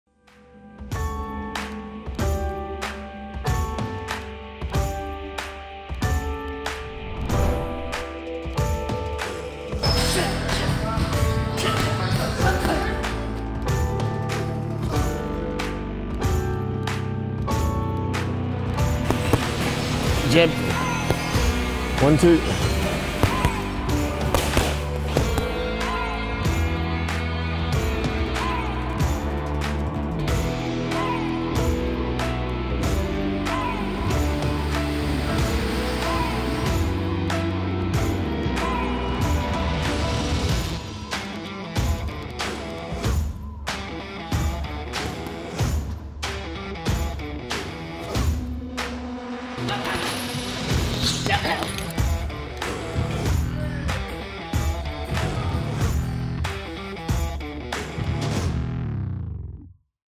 Genah Fabian X Engage I PFL Playoffs
Engage athlete, Genah Fabian ready to move up the PFL Playoffs in a can’t miss matchup with Kayla Harrison.